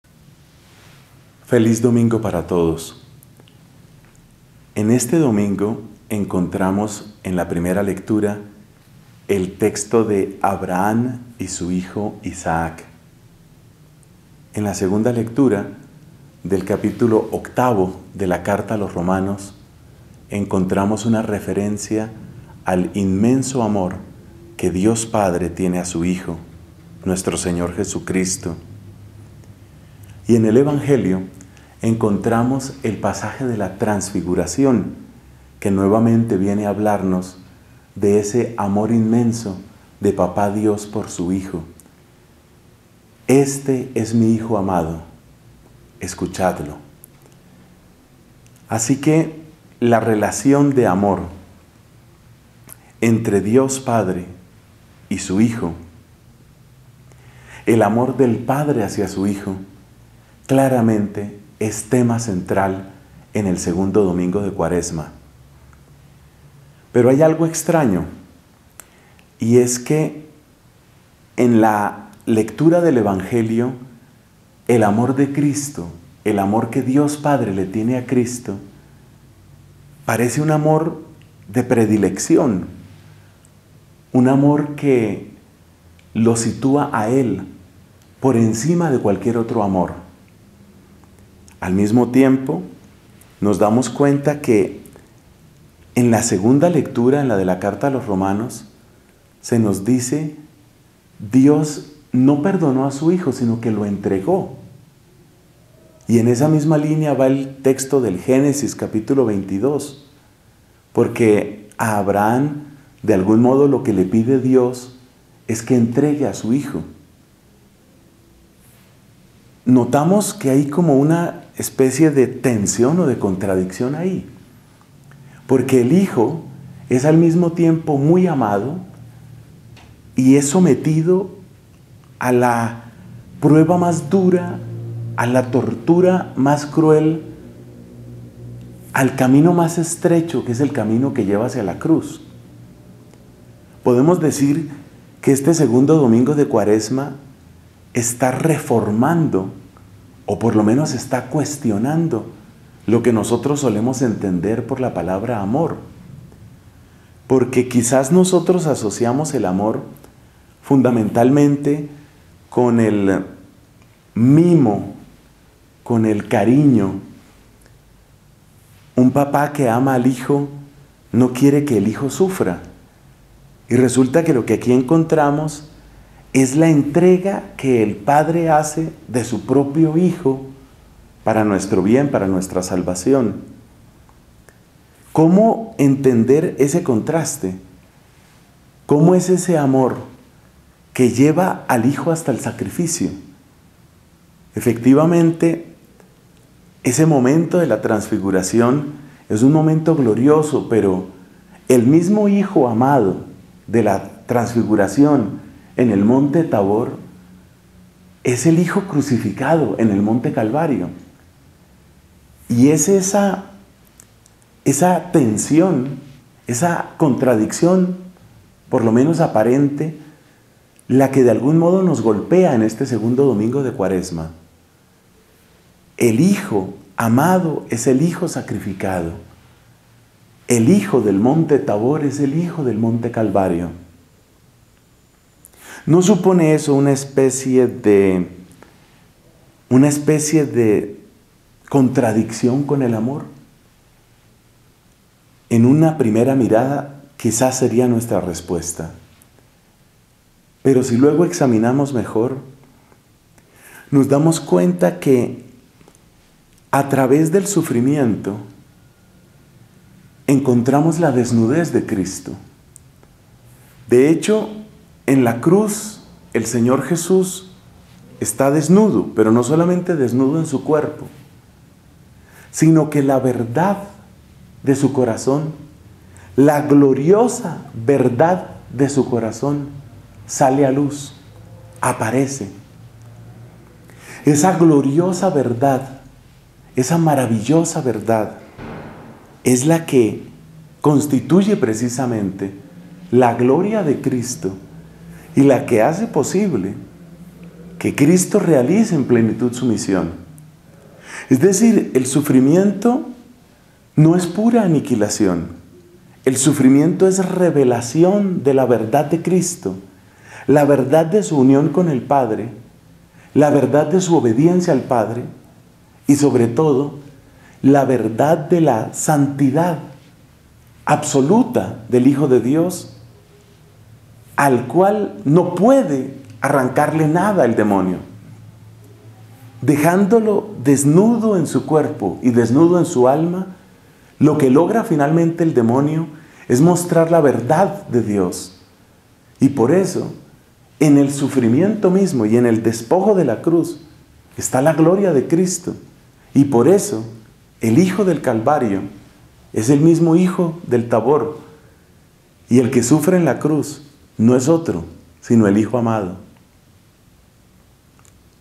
LA GRACIA del Domingo 1 de Marzo de 2015
DOMINGO II DE CUARESMA, CICLO B

El sufrimiento del Hijo parece extraño ante el amor del Padre; pero en ese sufrimiento se revela la verdad del Hijo y aparece su gloria.